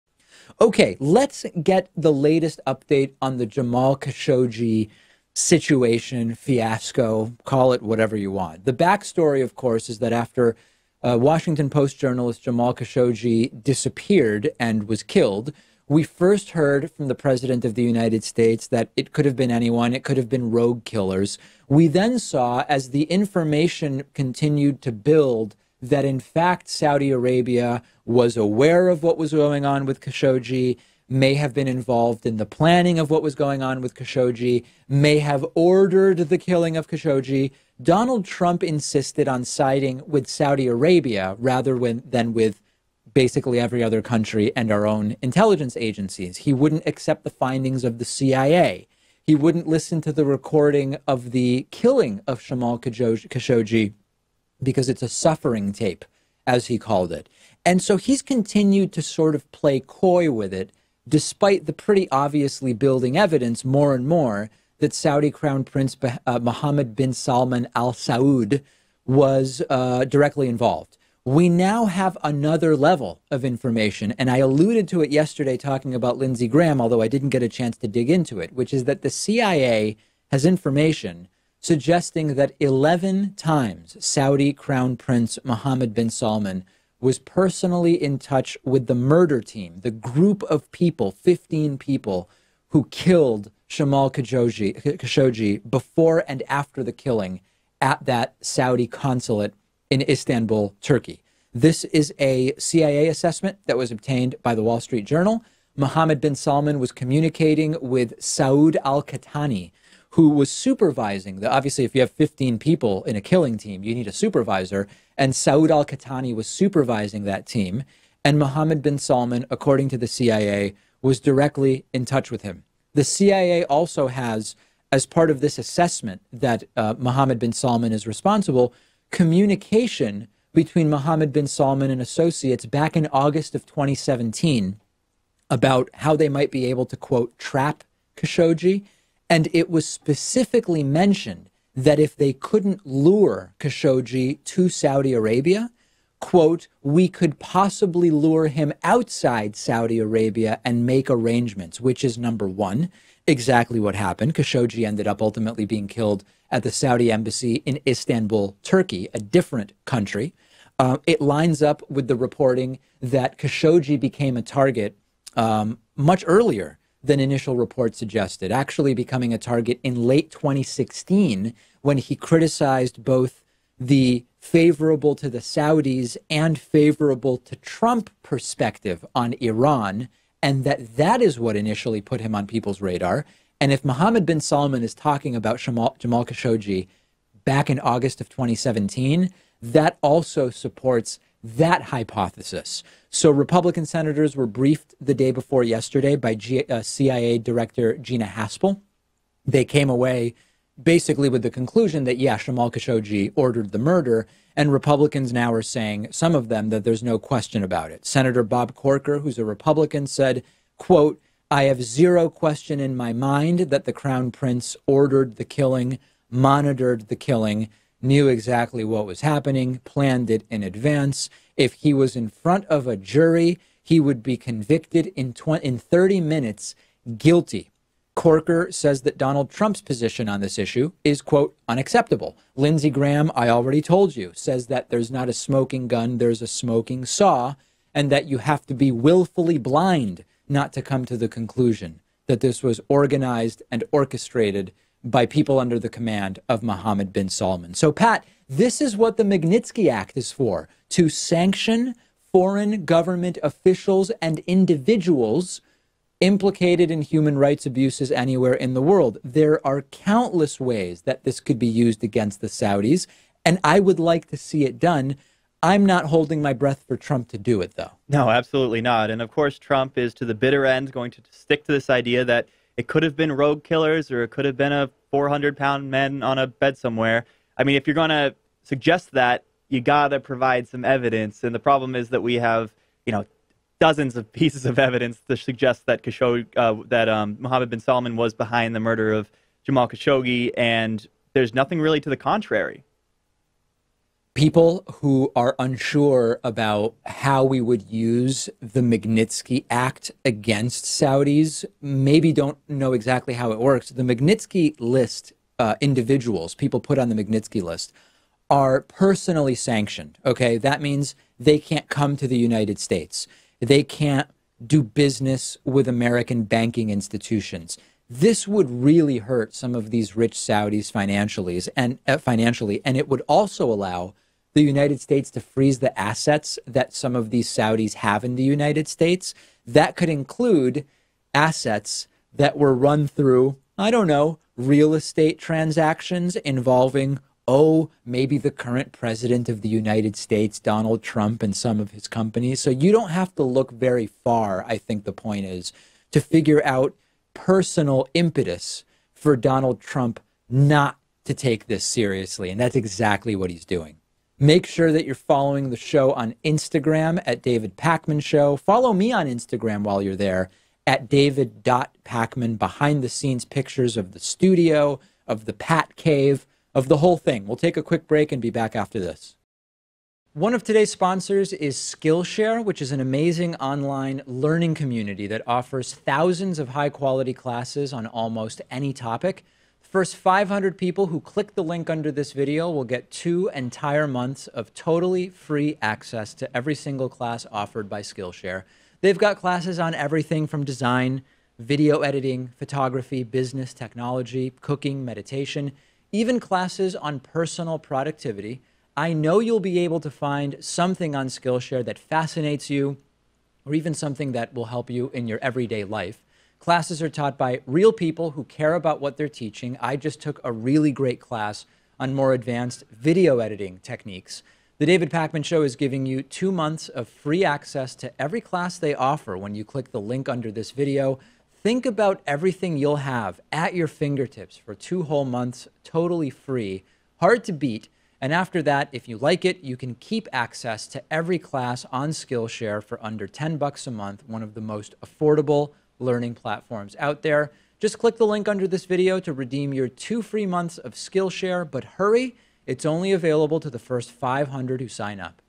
Saudi Crown Prince Sent 11 Messages to Jamal Khashoggi's Murderers
😃 Like FREE stuff?

--CIA evidence suggests that Saudi Crown Prince Mohammad Bin Salman Al Saud sent 11 messages to the supervisor in charge of the team that murdered journalist Jamal Khashoggi

Broadcast on December 6, 2018